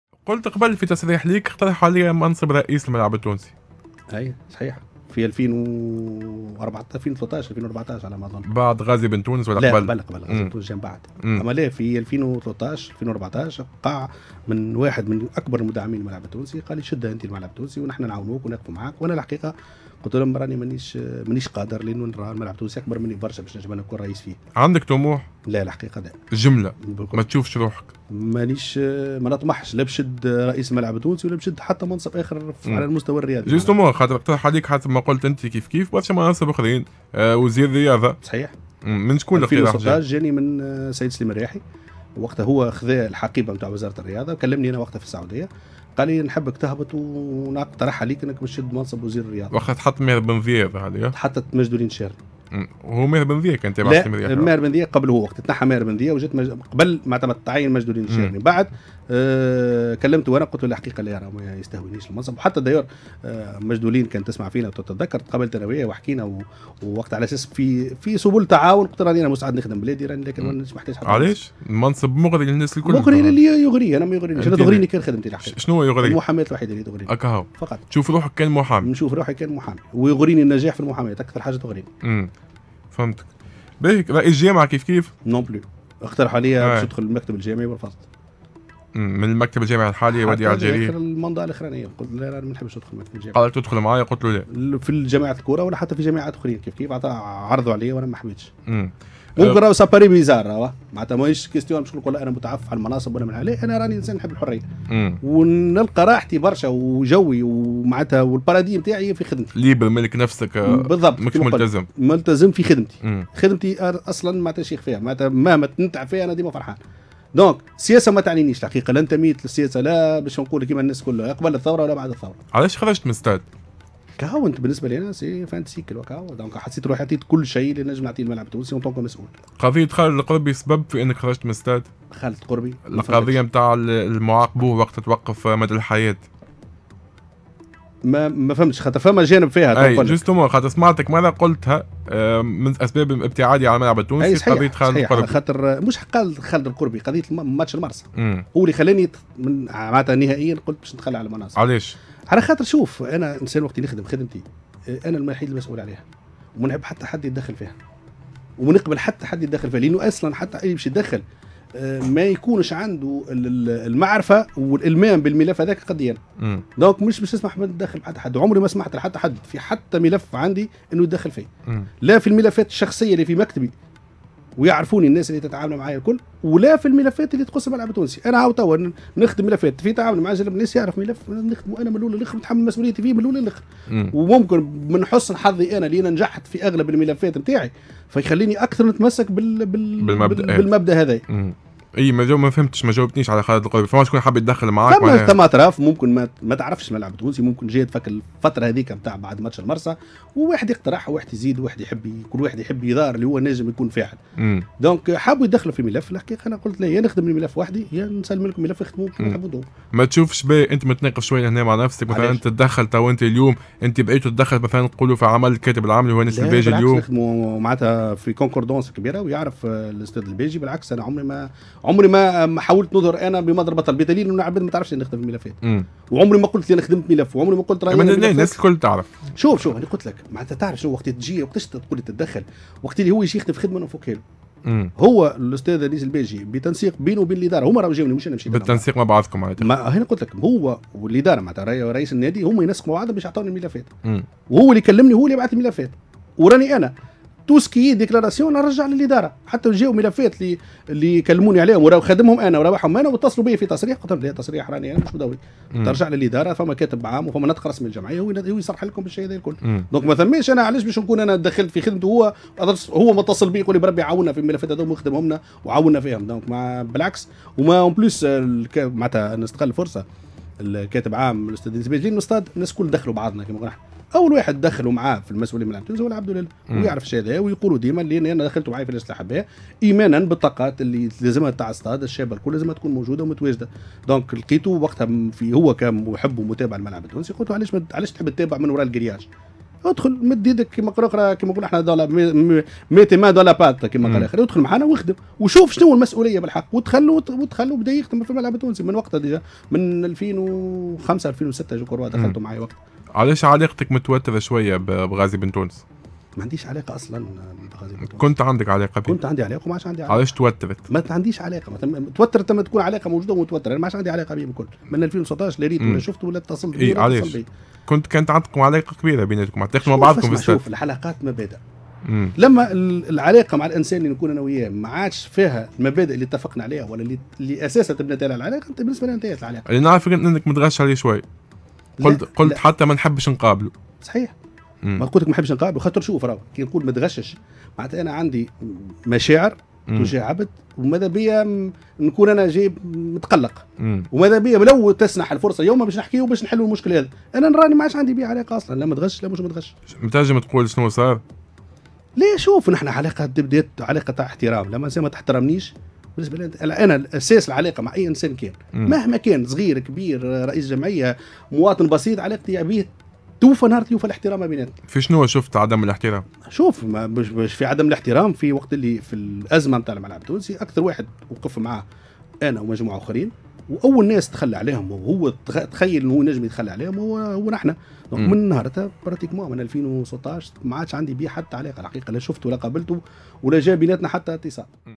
بن ميم:الرياحي اقترح عليا وزارة لرياضة والجري اقترح عليا الجامعة رفضتهم الزوز ورئاسة الستاد أكبر مني
أنيس بن ميم: سليم الرياحي اقترح عليا وزارة الرياضة ووديع الجري اقترح عليا الجامعة رفضتهم الزوز ورئاسة الستاد أكبر مني